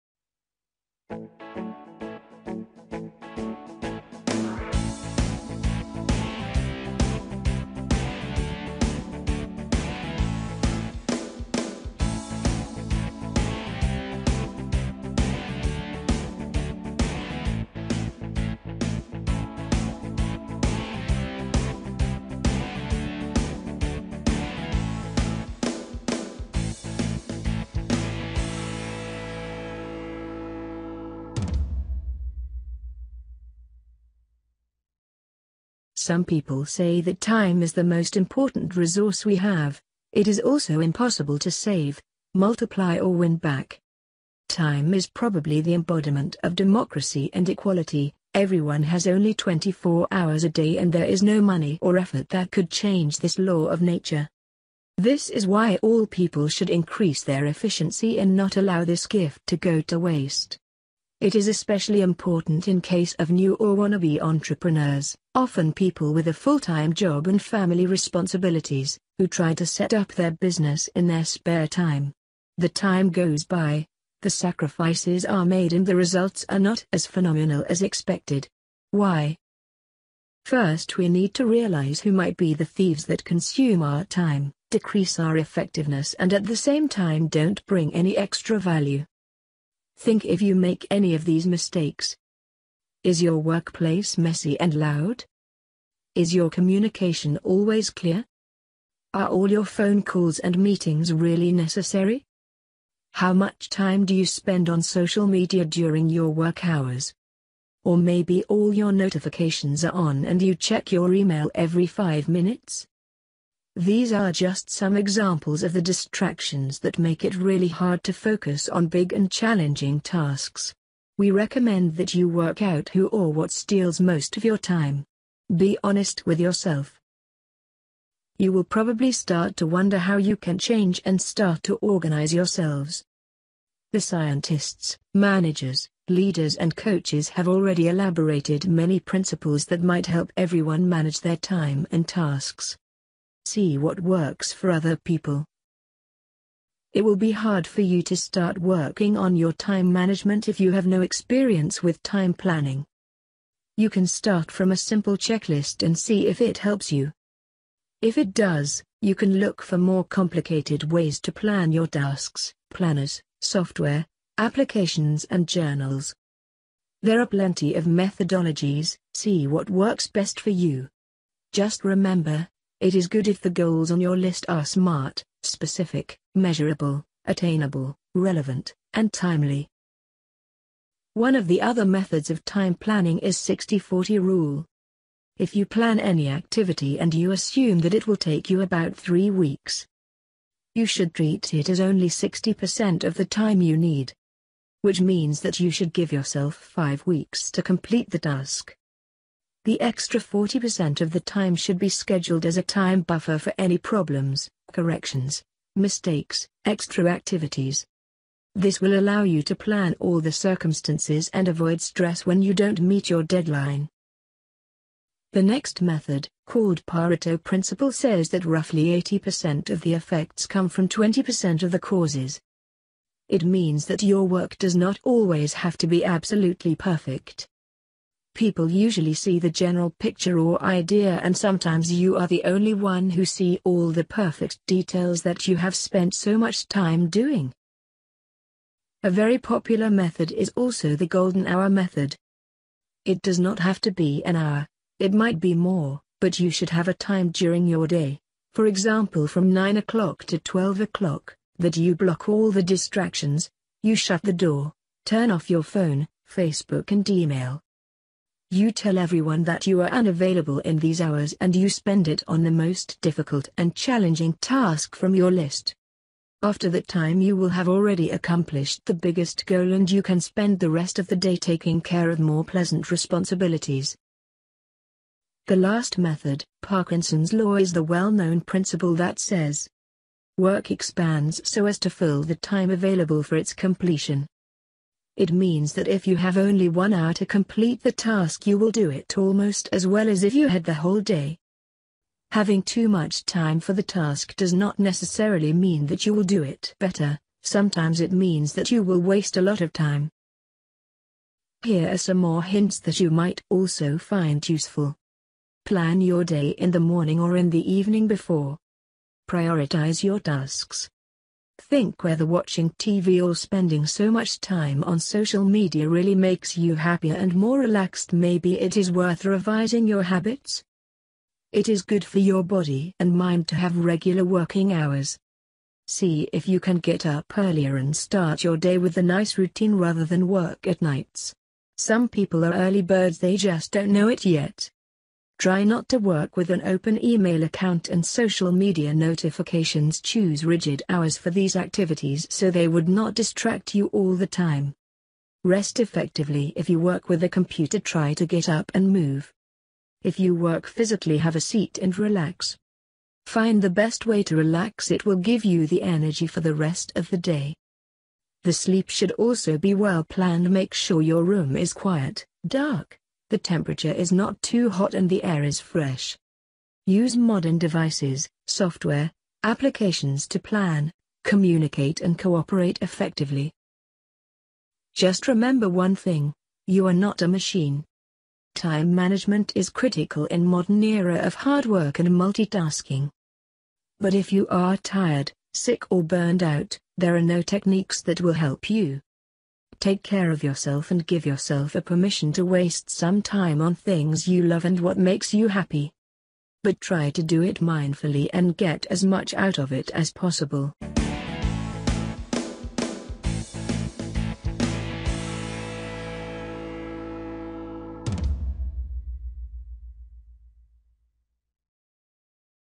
Időgazdálkodás
Mennyire gazdálkodunk megfelelően az időnkkel a hatékonyabb teljesítmény elérése érdekében?